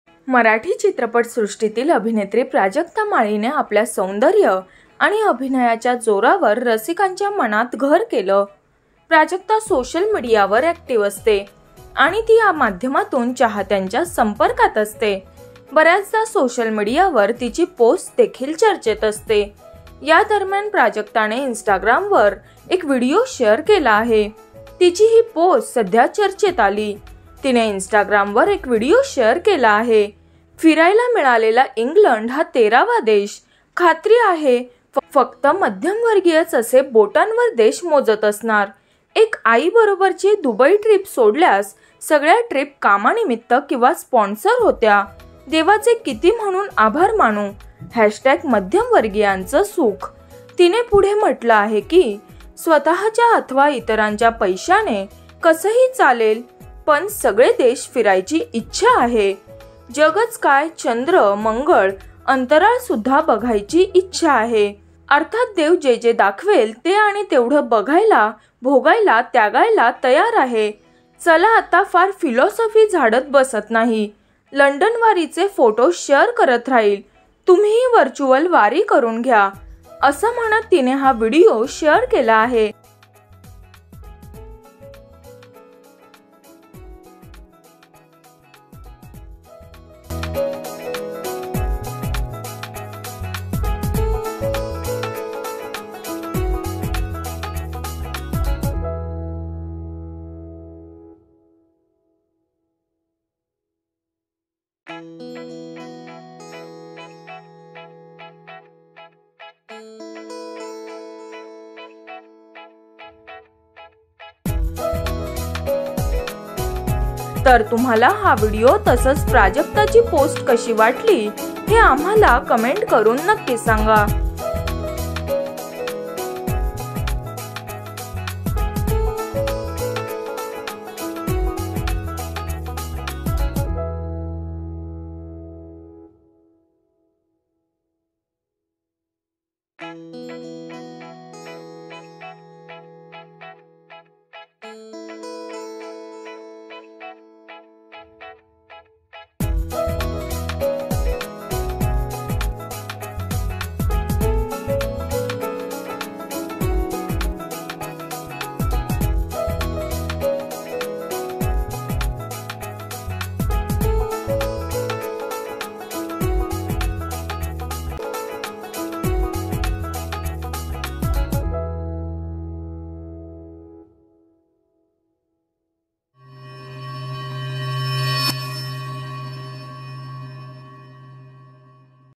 प्राजक्ताची तेरा देशांची हटके वारी | Prajakta Mali | Maharashtrachi Hasyajatra | Raanbaazaar
मराठमोळी अभिनेत्री प्राजक्ता माळीची तेरा देशांची हटके वारी,चला पाहुयात